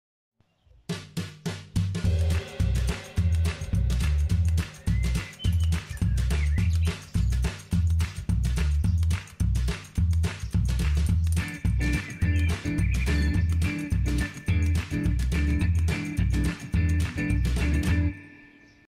You won't believe what this magician pulls from an EMPTY cup!
This video is crazy! After showing his cup empty, magician Gyula Szokor pulls something from it and you won't believe your eyes!

Original idea by Gyula Szokor, based on a magic effect by Alexander May.